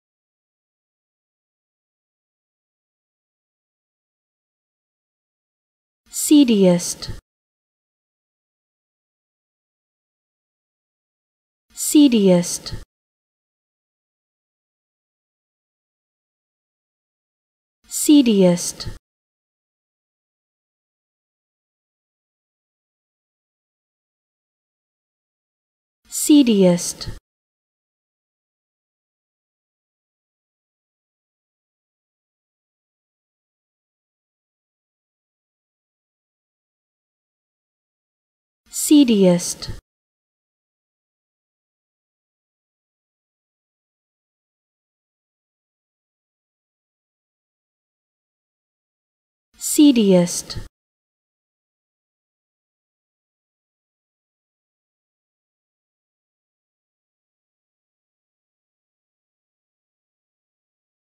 How To Say Seediest
Learn how to say Seediest with EmmaSaying free pronunciation tutorials.